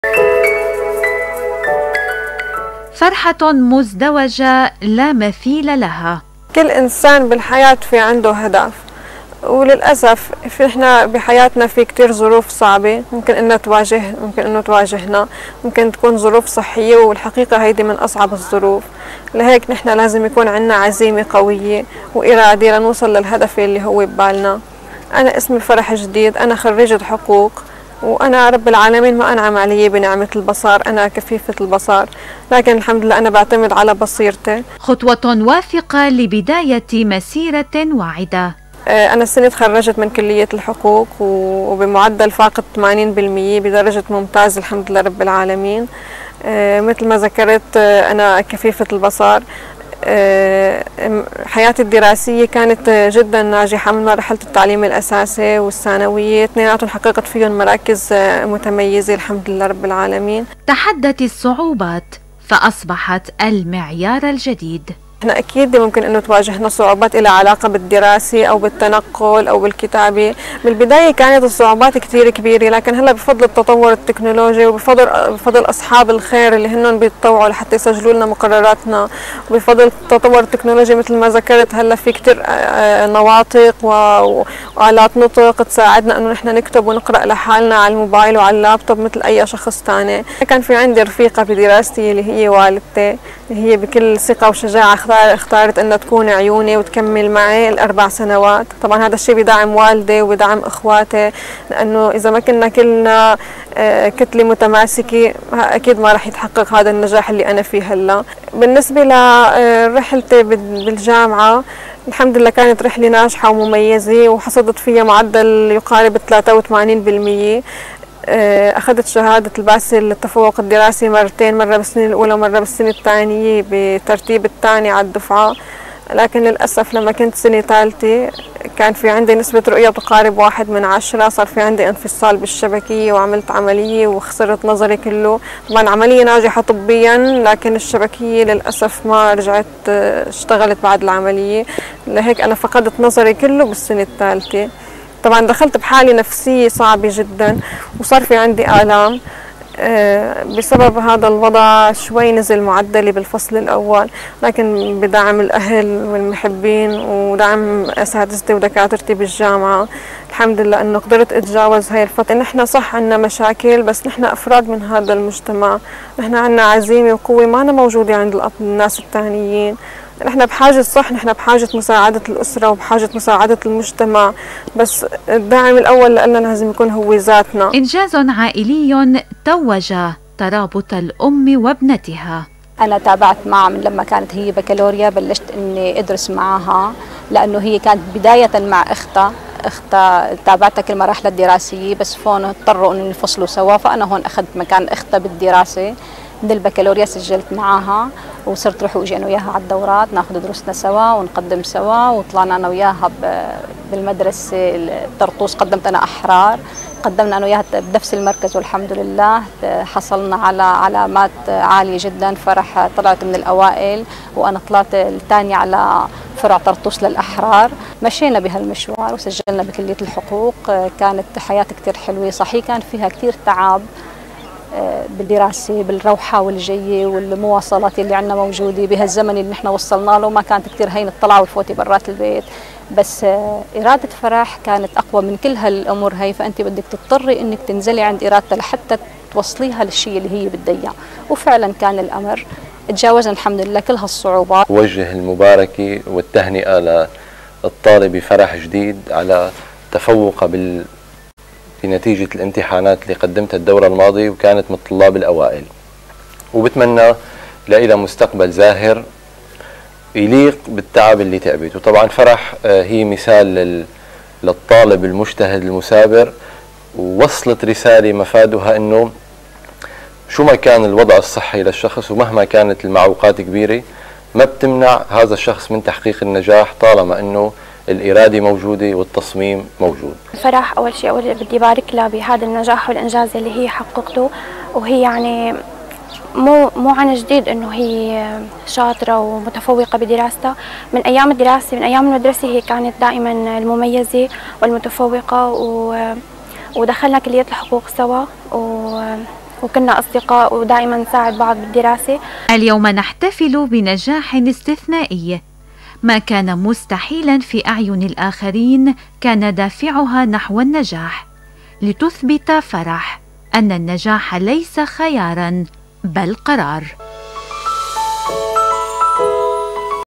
الطالبة فرح تتصدر كلية الحقوق رغم فقدانها للبصر
تقرير إيمان_عساف
المركز الإذاعي والتلفزيوني في اللاذقية
قناة اوغاريت